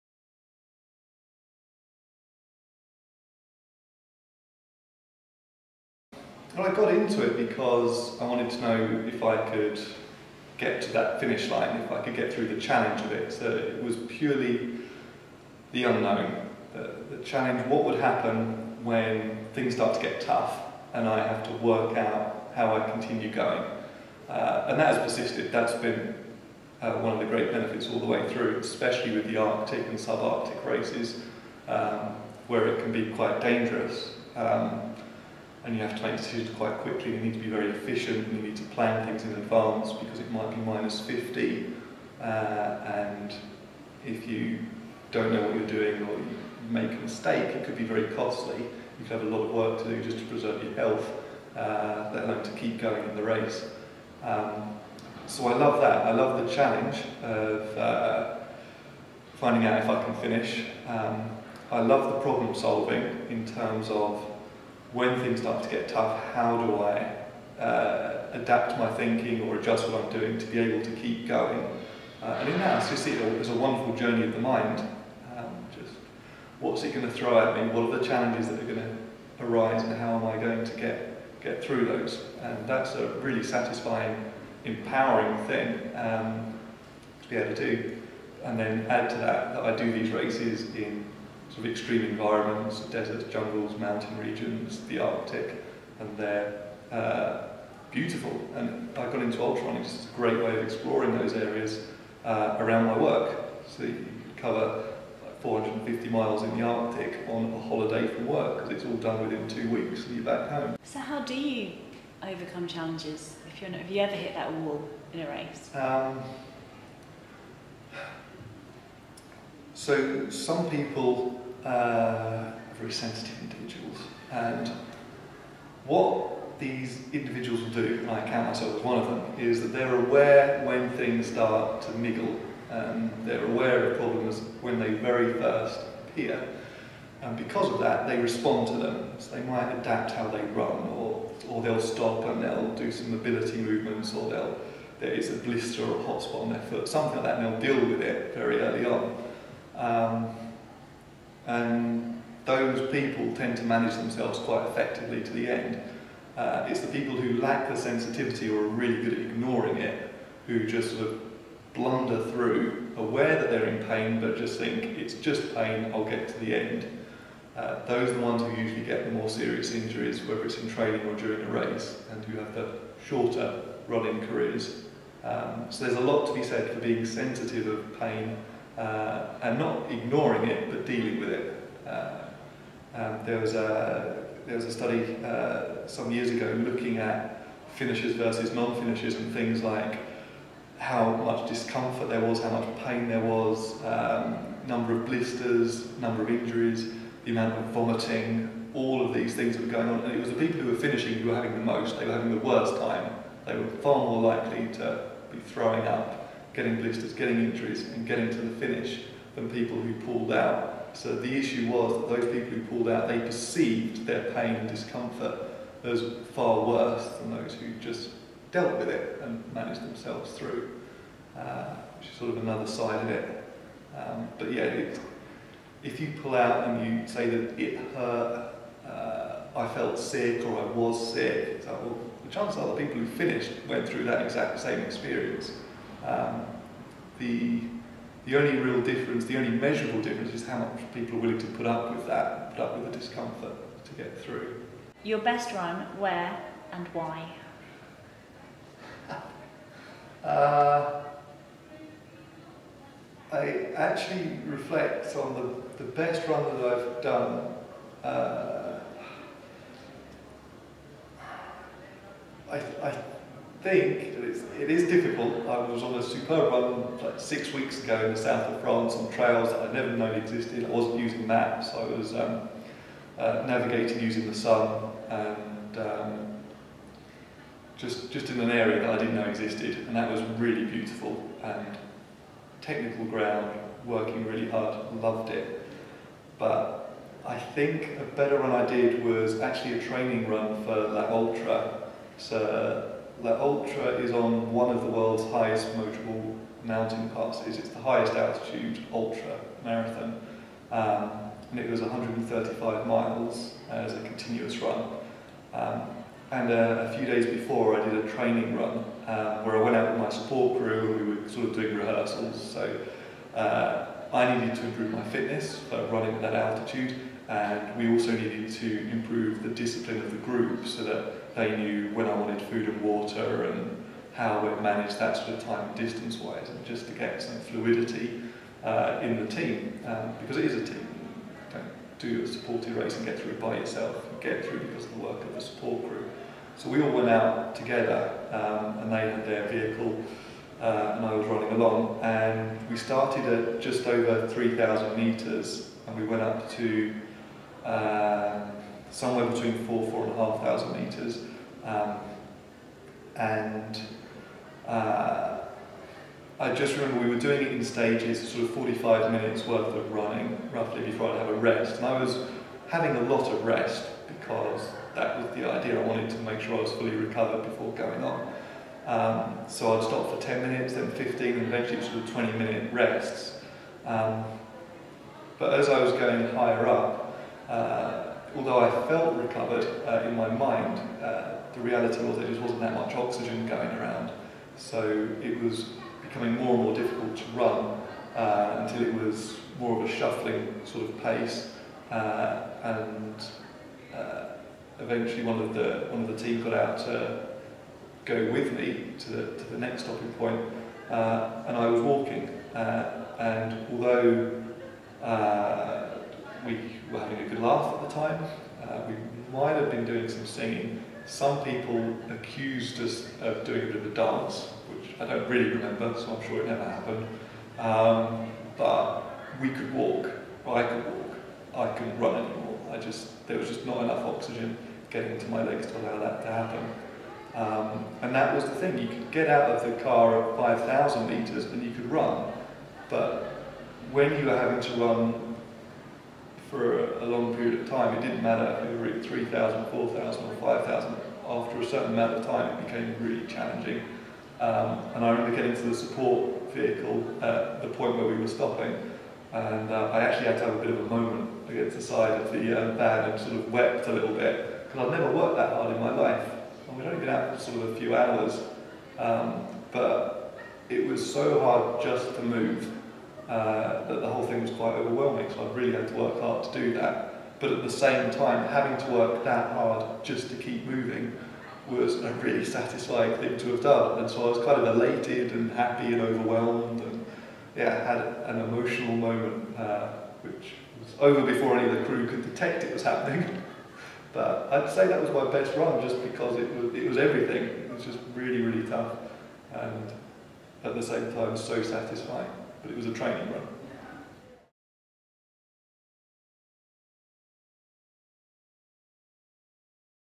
Mark Hines, Ultra Runner, Q&A Highlights
Mark Hines is an exercise physiologist and biomechanist who competes in the toughest ultra-endurance races in the world.  He has raced in rainforests, across deserts, over mountains, and in the sub-Arctic and Arctic.  He volunteered his time to hold a Q&A for MMTO members to help them (literally) follow in his footsteps!

To watch the full Q&A, join for £1 ONCE and get involved in the new charity dream swap!